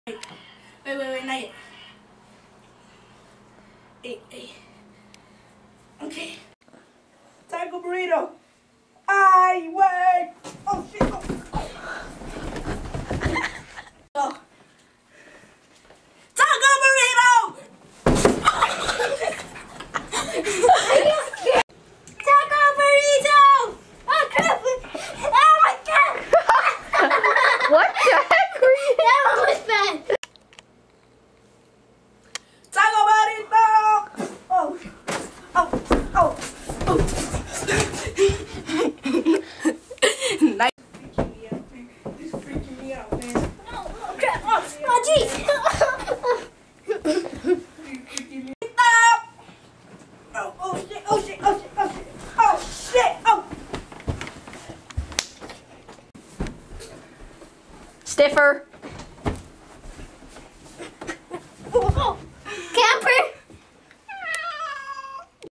taco bloopers (PG-13)
From the creator of the taco burrito videos, here is a collection of our biggest, most painful, most swear filled mess ups, accidnts and ultimate flops. This is taco bloopers, the biggest bloopers from the taco burrito world crammed into one PG-13 rated video. Here is the link to taco burrito 1 and 2
   (PG-13: some language and a painful stunt)